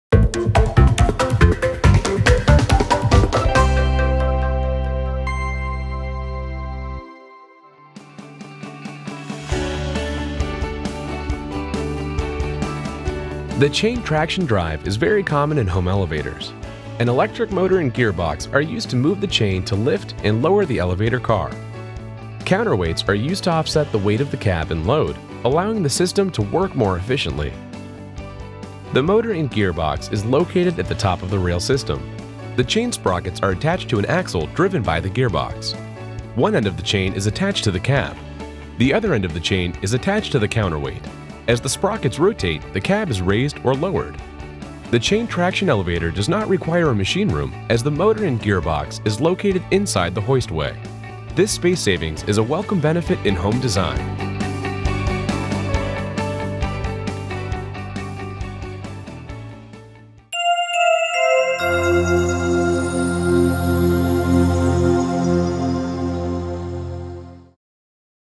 Chain Traction Home Elevator: How do elevators work? | Nationwide Lifts
This 3D animation demonstrates the operation of a chain traction home elevator.

The chain traction drive is very common in home elevators. An electric motor and gearbox are used to move the chain to lift and lower the elevator car. Counterweights are used to offset the weight of the cab and load, allowing the system to work more efficiently.

to download the free brochure and as for a free quote